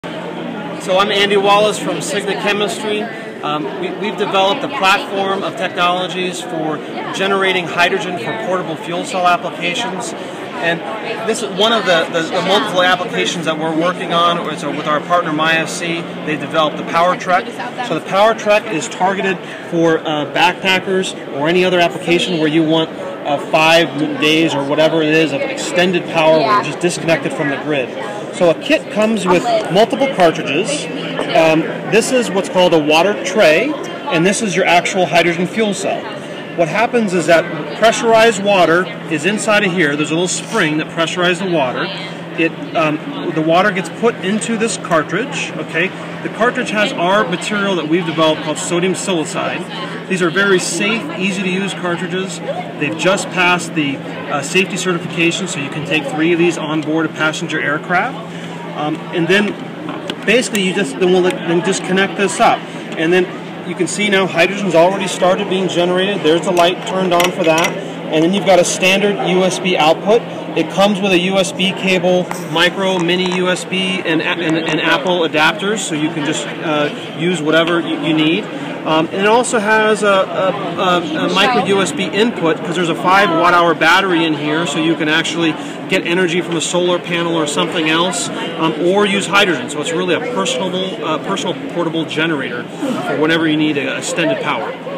PowerTrekk powers gadgets with hydrogen
Manhattan-based Chemistry provides eco-friendly hydrogen fuel cells that can power anything, even laptops and refrigerators. This portable solution is just for mobile devices and gives you up to 4 hours of energy with a single 12$ cartridge (while the charger itself is 200$).